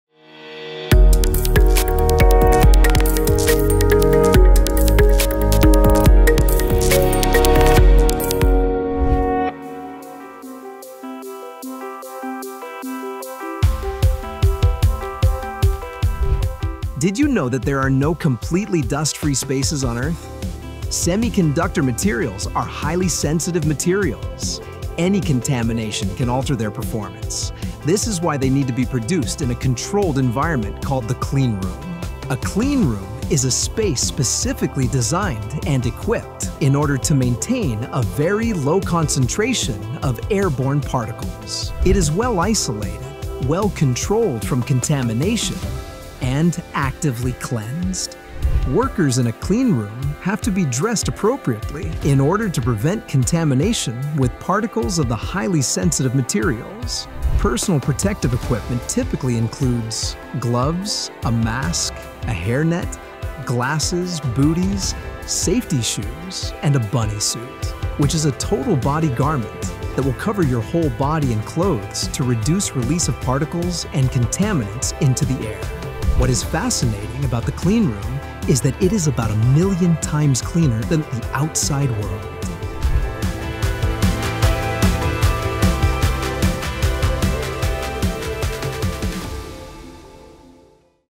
Material Innovation 101: Cleanrooms
🎥 Grab your favorite snack and join us for the latest Material Innovation 101 video to learn just how clean a cleanroom is and why it’s so important to the precise semiconductor processes we enable.